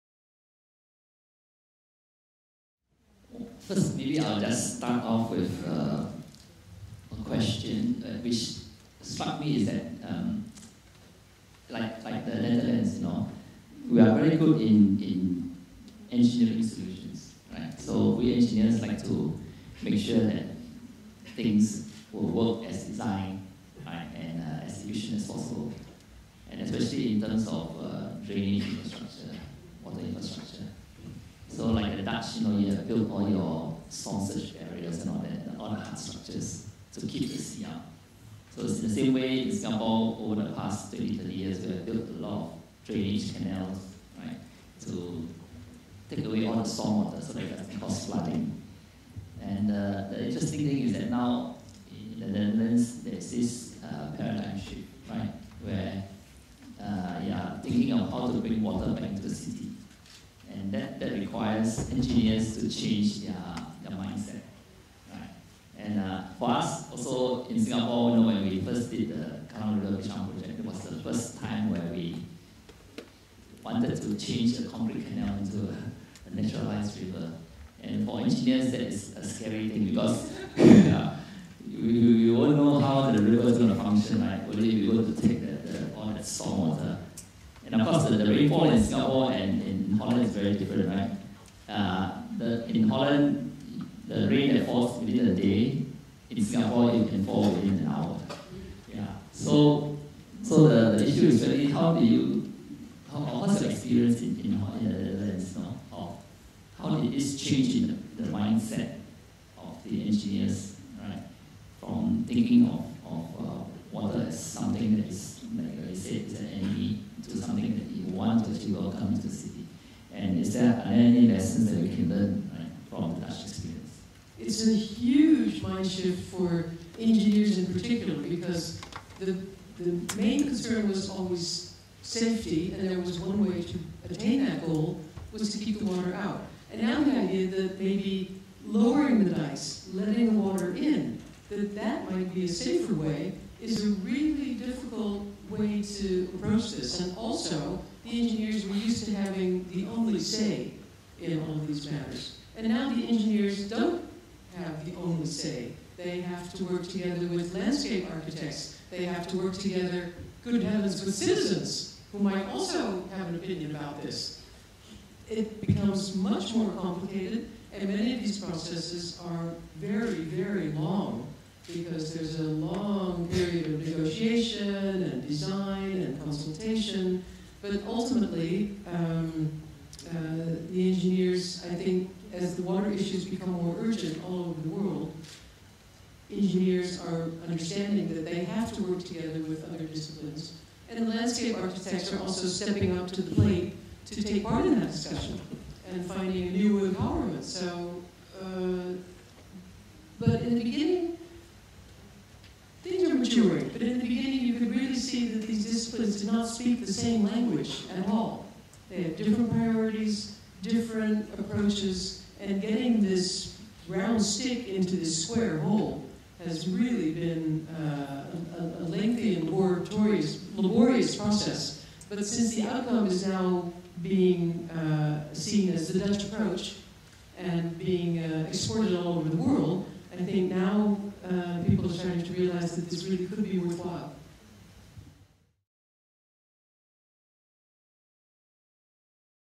Tracy Metz: Engineering change in urban design
This is a snippet from the CLC lecture 'Water - Holland's "Frenemy" by Tracy Metz, Director of the John Adams Institute and author of the book Sweet and Salt: Water and the Dutch.